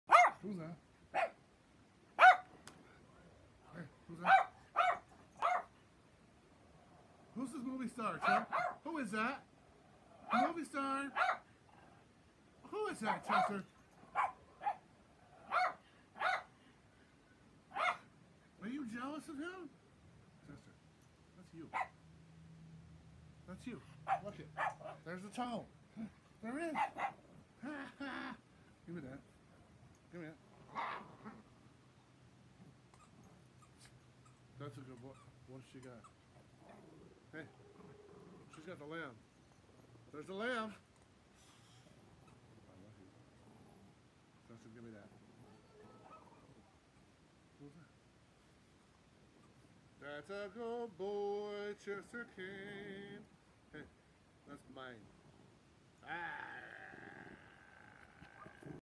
Chester watching himself on YouTube
Rat terrier Chihuahua mix